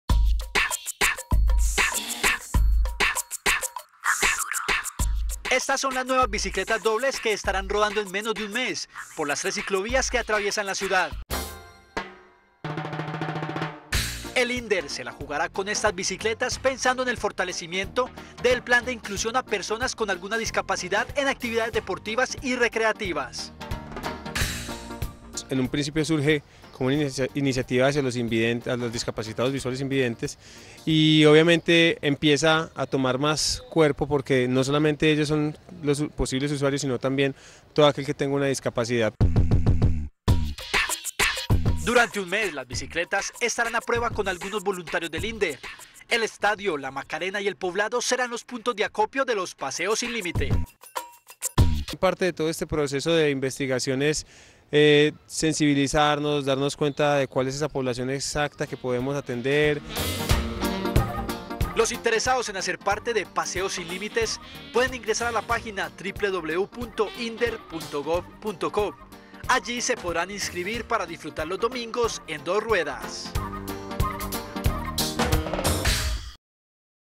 Inder estrená bicicletas para discapacitados
El Inder estrenó en la ciclovía del estadio las nuevas bicicletas para personas con discapacidad, gracias al programa Paseos sin Límites y a Comfenalco Antioquia los deportistas podrán acceder a este servicio.